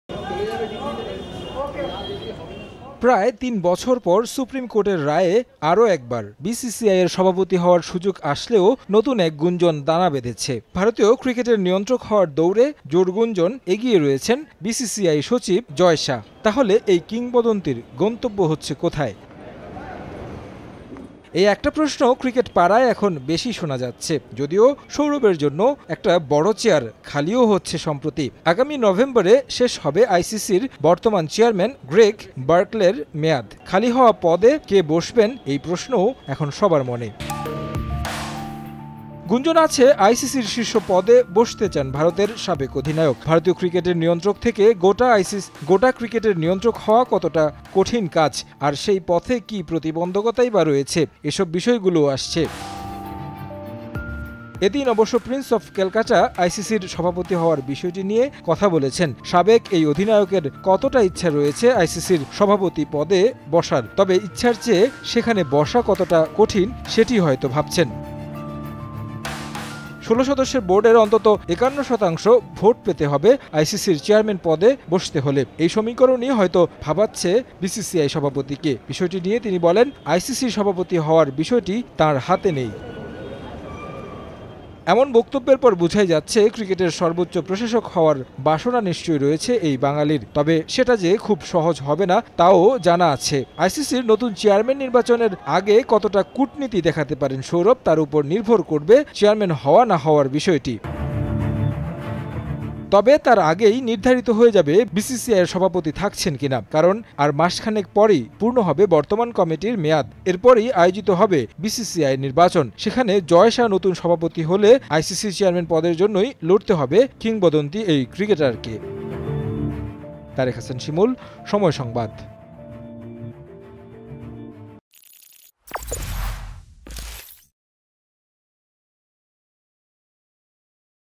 আইসিসির সভাপতি হওয়া আমার হাতে নেই: গাঙ্গুলি | Sourav Ganguly | Jay Shah | BCCI | ICC | Somoy TV
আইসিসি'র পরবর্তী চেয়ারম্যান হবেন বিসিসিআই প্রেসিডেন্ট সৌরভ গাঙ্গুলি। এমন গুঞ্জন শোনা যাচ্ছে ক্রিকেট মহলে। তবে ক্রিকেটের নিয়ন্ত্রক সংস্থার চেয়ারম্যান পদে বসার বিষয়টি তার হাতে নেই বলে জানিয়েছেন প্রিন্স অফ ক্যালকাটা। নতুন মেয়াদে জয় শাহ বিসিসিআইয়ের প্রেসিডেন্ট হলে গাঙ্গুলিকে দেখা যেতে পারে আইসিসি'র চেয়ারম্যান পদে।